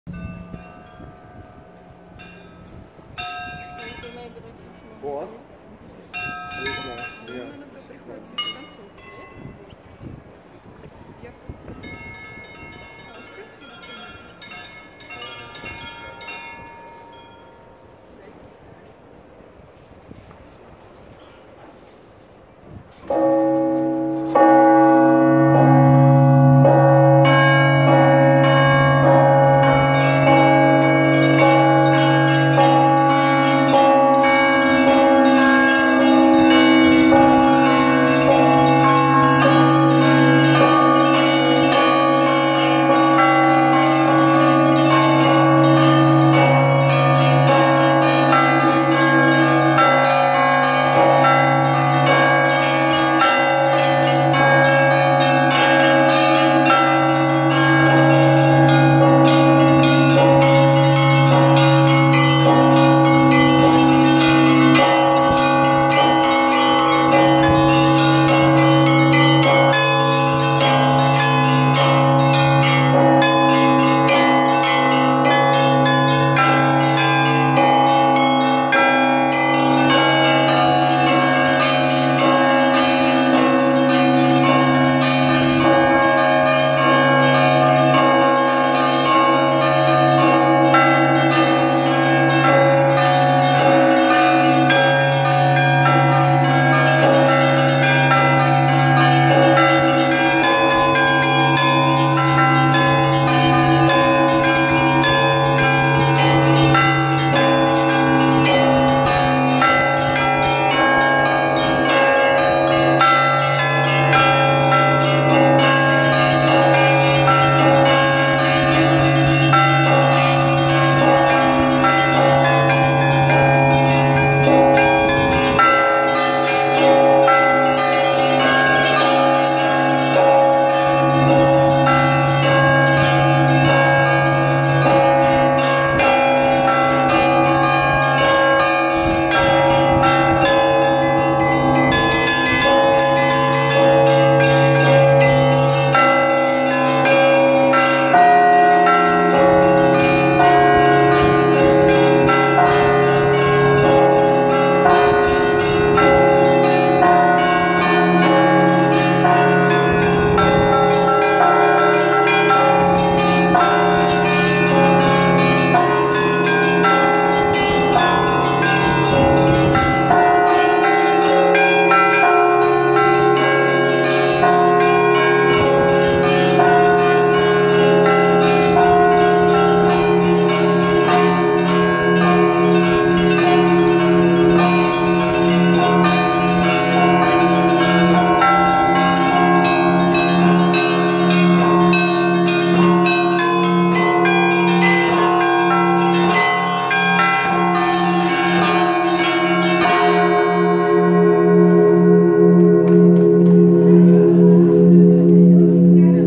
Сергей Грибанов фестиваль "Заиконоспасская колокольня" 19.12.2020
Звонарь Сергей Грибанов колокольный звон на фестивале "Заиконоспасская колокольня" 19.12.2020 в Заиконоспасском монастыре.